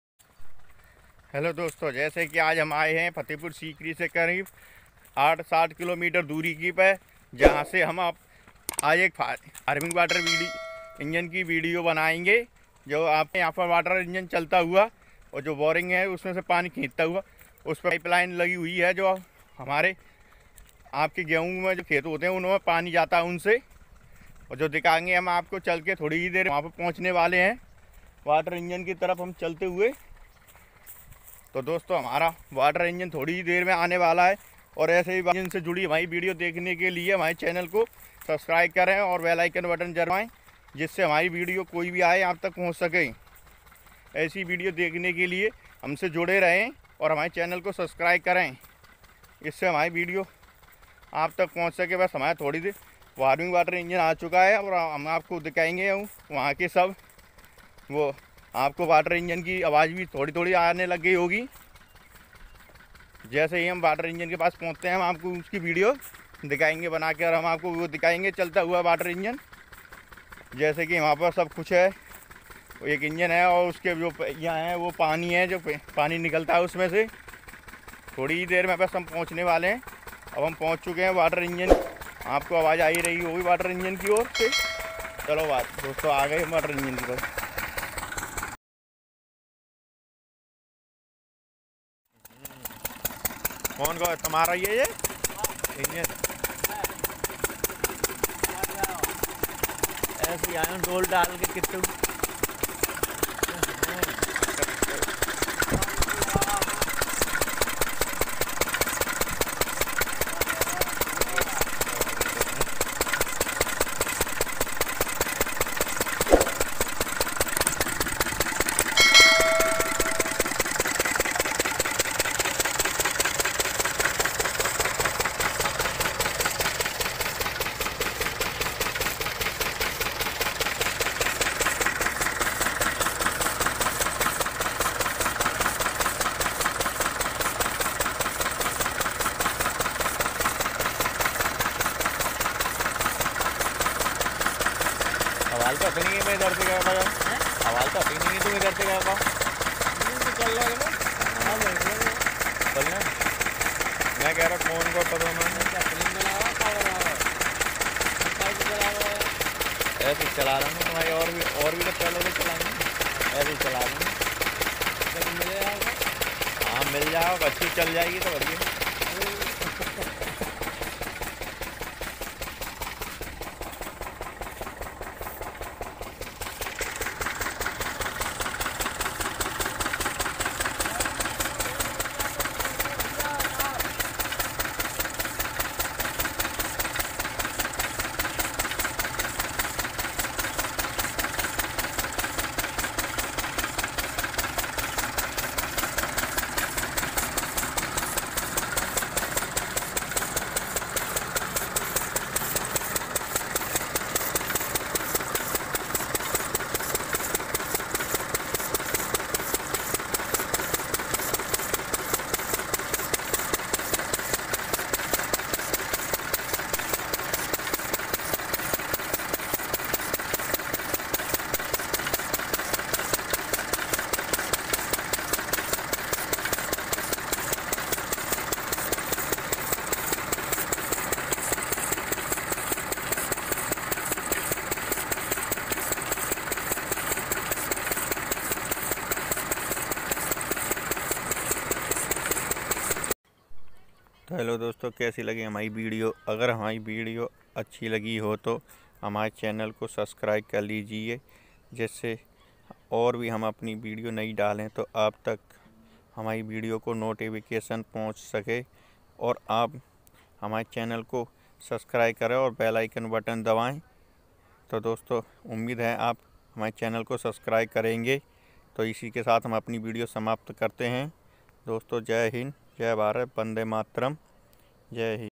How to make farming water diesel engine 2020/Siani all India
हेलो दोस्तों जैसे कि हम अपनी आज की नहीं वीडियो में दिखाएंगे आपको डीजल वाटर इंजन की वीडियो इस वीडियो में आप देखिए फार्मिंग का बोल वाटर डीजल इंजन होता है